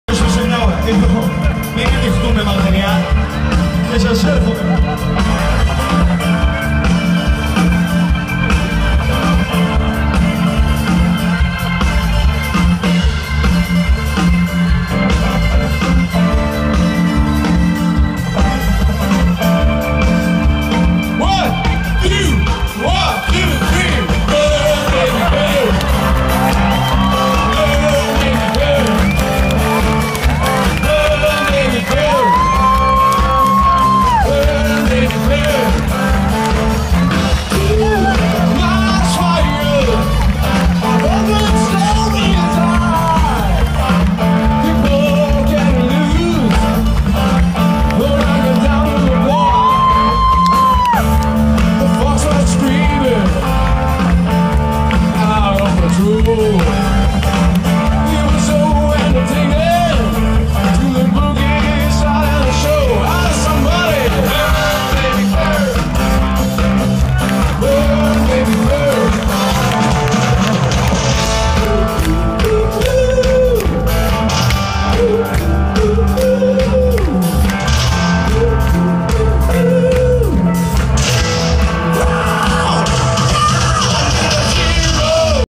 matala 2014 tonis sfinos flying dolphin
ο τονυ ίπταται στο φεστιβάλ Ματάλων 2014